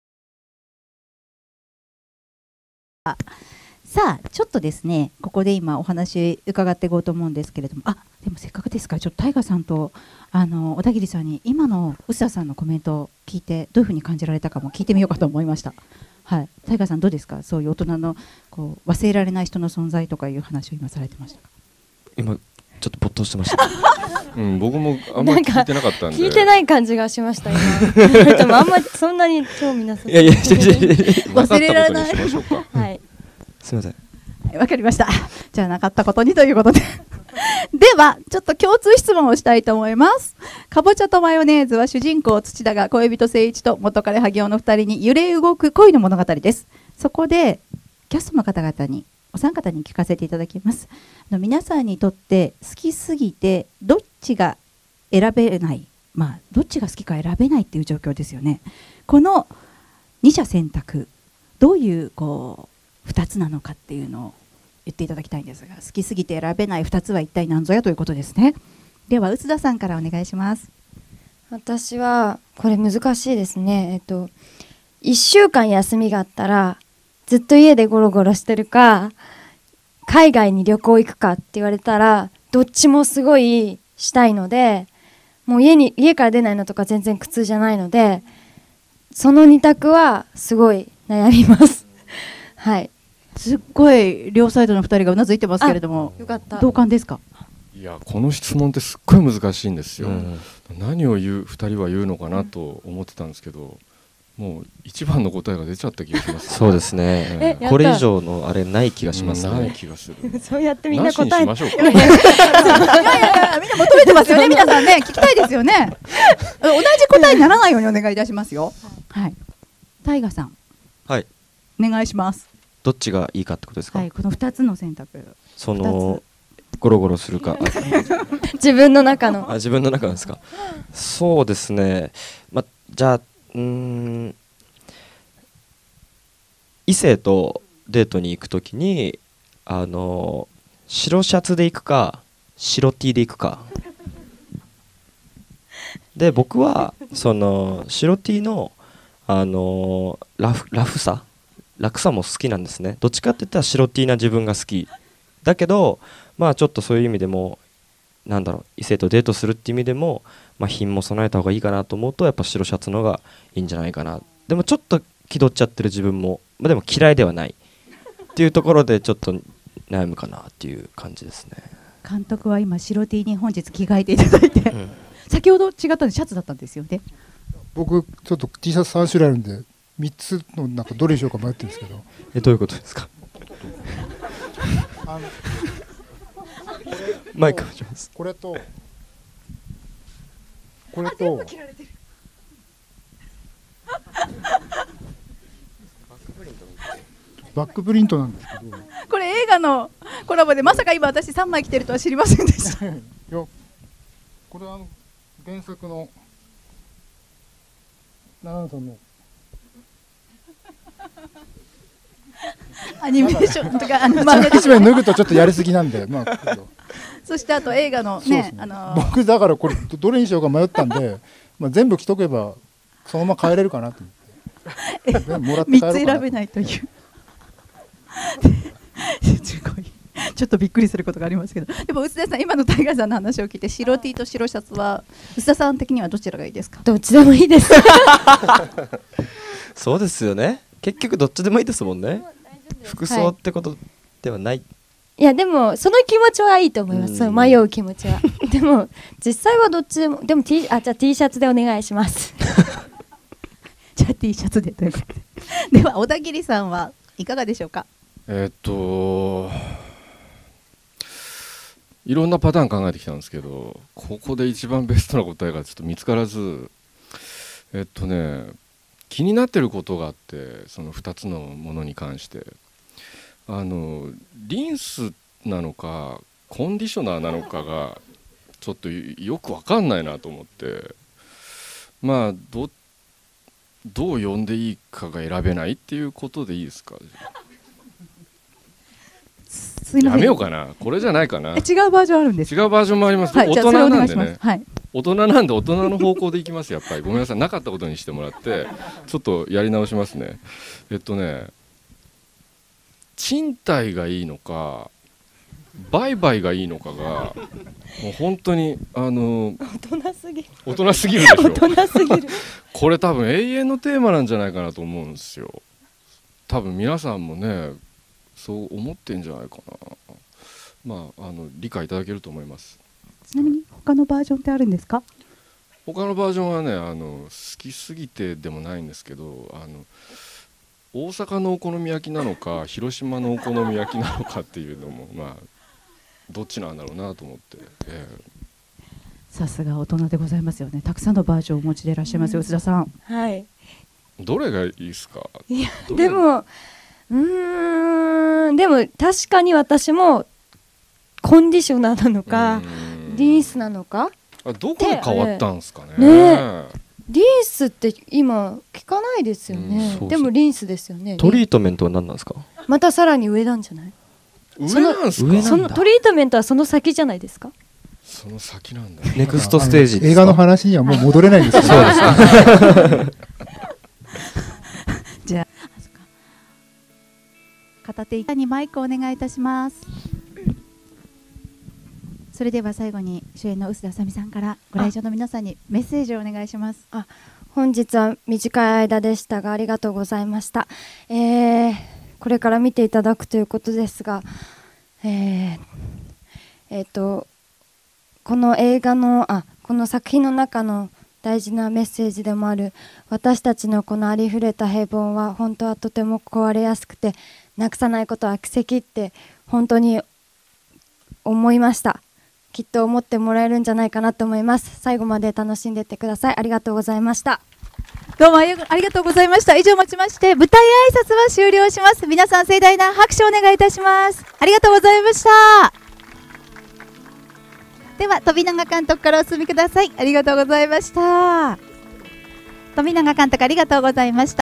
オダギリジョー、リンスとコンディショナーで悩む？臼田あさ美も“共感” 映画「南瓜とマヨネーズ」完成披露上映会3
女優の臼田あさ美さんが10月10日、東京都内で行われた主演映画「南瓜とマヨネーズ」（冨永昌敬監督、11月11日公開）の完成披露上映会に共演の太賀さん、オダギリジョーさんとともに登場した。

　今作は、マンガ家・魚喃キリコさんの代表作を実写化。生活費を稼ぐため、密かにキャバクラで働く女性ツチダ（臼田さん）が、夢を追い続けるミュージシャン志望の恋人、せいいち（太賀さん）と、自由奔放で女好きな元彼のハギオ（オダギリジョーさん）の間で心が揺れ動く……という物語。